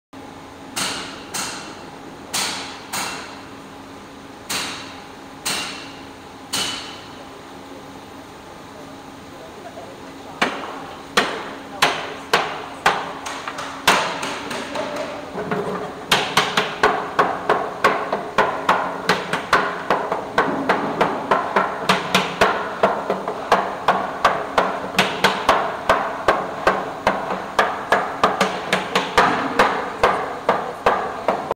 How many staves in a barrel? - Wine Tok - Napa Valley Wine Academy
Here it is👇
💾 But first, LIKE & SAVE this wine knowledge so you can come back to it!
How many staves are needed to make a standard wine barrel ?🤔
Staves are the flat wooden sticks that make up the body of the barrel. Every barrel has around 28-34 staves, which are arranged in a specific pattern. The setup is a puzzle, as each stave varies slightly in width, and a barrel cannot have any gaps.

Now you know! FOLLOW @napavalleywineacademy or more wine knowledge and wine study tips